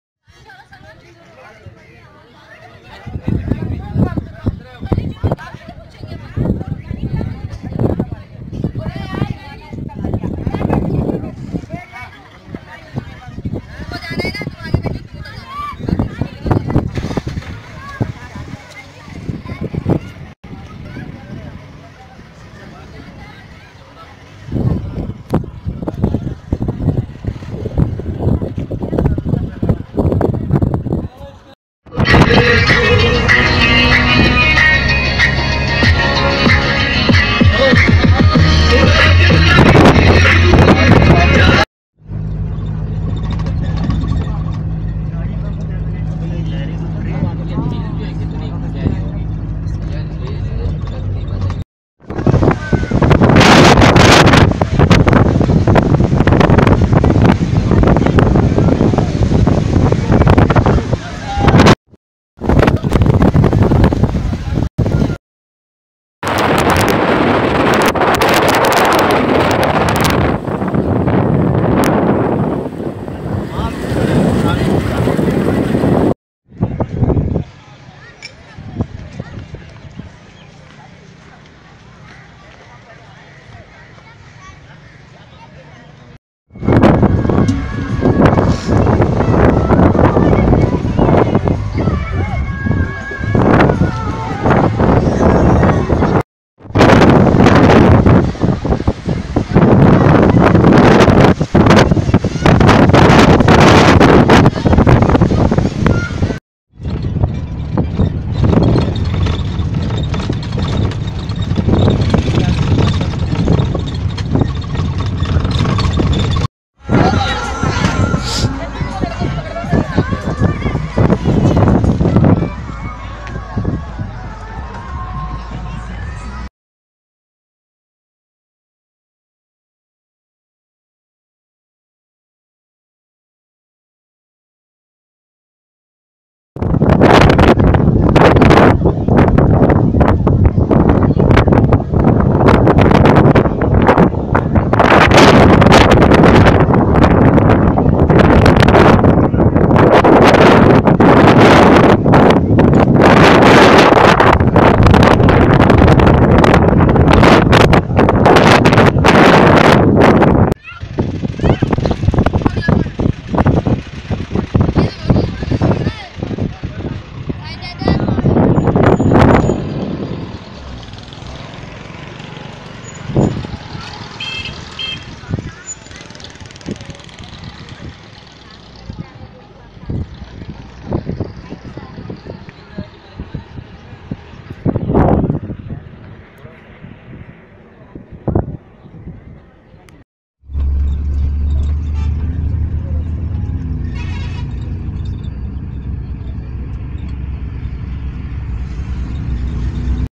Makli Thatta | Keenjhar Lake | Badsha Masjid | Picnic BY FARAZ BAIG
Video Tital : Makli Thatta | Keenjhar Lake | Badsha Masjid | Picnic  BY FARAZ BAIG